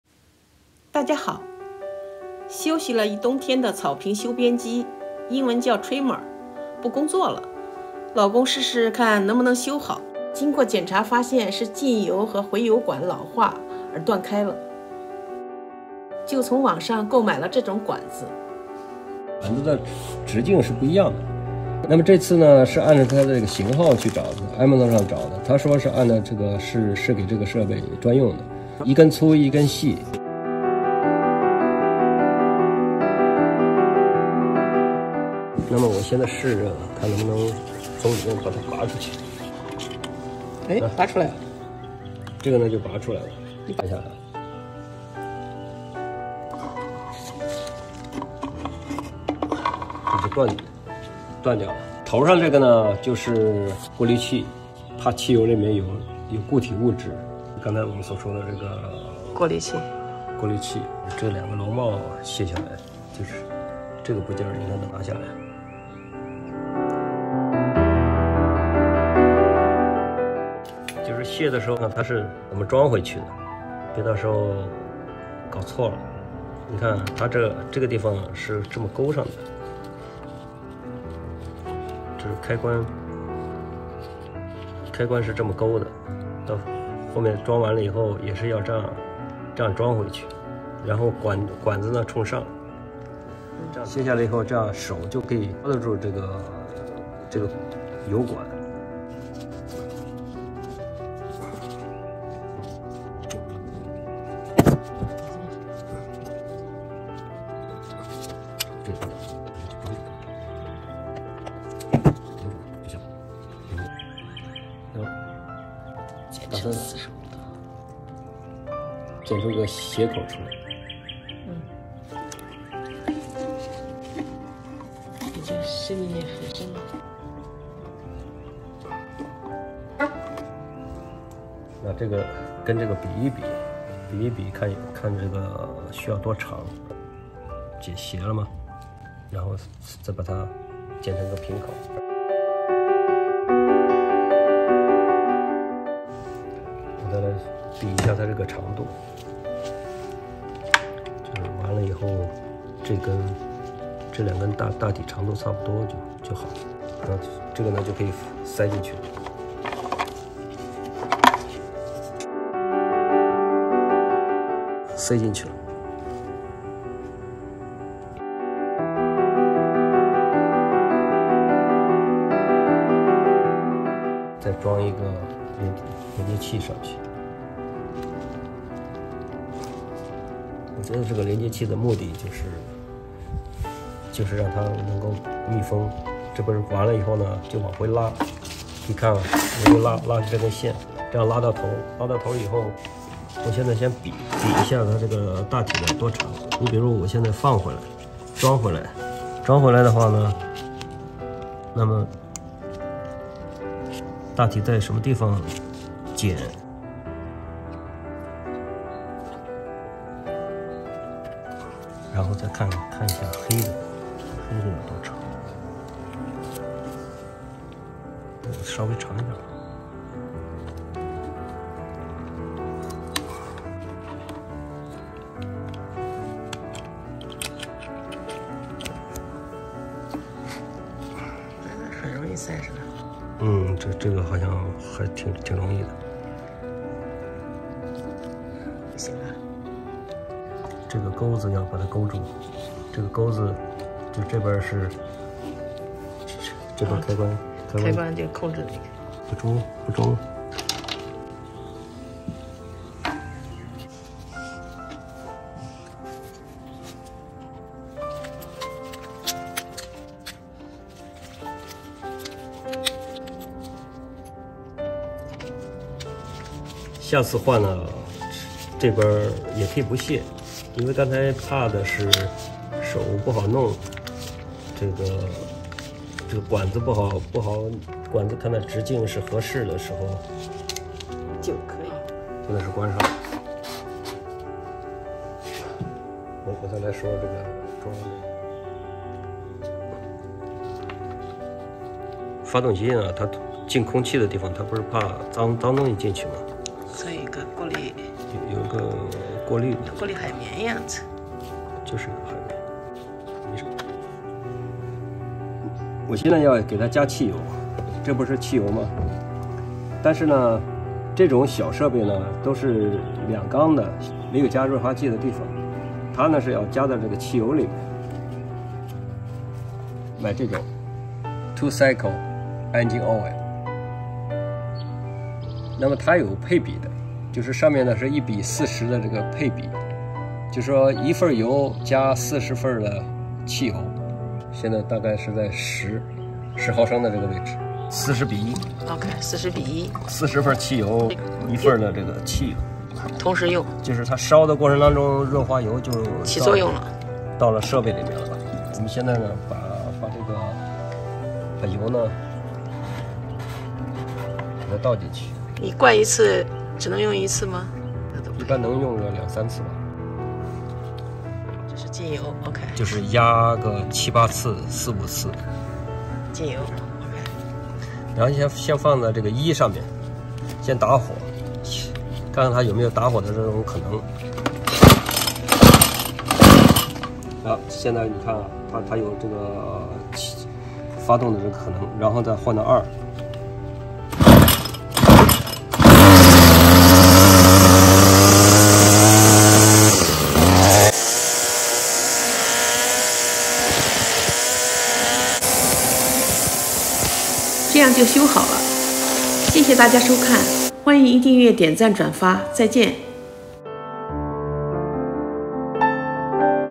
打边机坏了 怎么换油管？为啥润滑油要和汽油混一起？How to Change the Tubing Pipe When the Trimmer is Broken?
休息了一冬天的草坪修边机，英文叫Trimmer 不工作了，老公试试能不能修好
草坪打边机不工作了
检查发现是进油和回油管老化了
从网上买了这种管子
检查发现是进油和回油管老化而断开了
就从网上购买了这种管子
这两根管子直径是不一样的
这次是用型号去Amazon 上找的
打开盖看看里面
把断的管子拔出来
再拔出来另外一根
头上这个是过虑器
用于过滤气油里的固体物质
拿下顶部过滤海棉
再来拆这个部件
把螺丝拧下来
卸的时候要记住怎么装回去
这样就容易抓住油管
容易把断的塑料油管弄下来
两根旧的就拆下来了
再准备装新的塑料油管
为了好装进去 头部剪45 度
把油管装进油桶再拉出来
跟旧的比一比看需要多长
把斜的剪平
装上一个新的汽油过滤器
再与旧的比一下塞去汽油桶里的长度
然后塞进去
再弄另外一根
这一根也塞进去了
弄出来剪平装连接器
从油桶口把管子弄出来
把前面的斜口剪平
再装一个连接器为了密封
把两根管子从油桶侧孔往回拉
先比一下需要多长
大体在什么地方
剪成需要的长度
再看黑管需要多长
剪成需要的长度
很容易的就装上了
按原样装回去
装一个新的发动机的海棉过虑器
打边机装好了
下一步要加汽油
这个机器也需要有润滑油维护
机器小没有加润滑油的地方
需要与汽油混合加入
买的润滑油是这种
与汽油的比例是1；40
取十毫升润滑油
加四十毫升汽油
在量杯中混合
汽油在发动机里燃烧中润滑油也到了设备里了
把油倒入打边机油桶
按操作步骤启动
成功了！